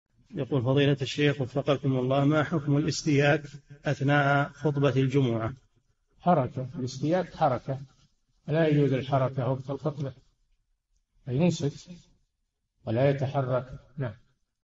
[3039- 4717] ما حكم الاستياك أثناء خطبة الجمعة؟ - الشيخ صالح الفوزان
[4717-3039] ما حكم الاستياك أثناء خطبة الجمعة؟
الجَوَابُ: حركة الاستياك حركة، ولا يجوز الحركة وقت الخطبة، بل ينصت ولا يتحرك.
السيرة الذاتية :
للاشتراك في قناة الشيخ صالح الفوزان
لمشاهدة حلقات فتاوى على الهواء كاملة

للاستفسار والإقتراحات والدعم
رئيس الفريق
لمشروع كبار العلماء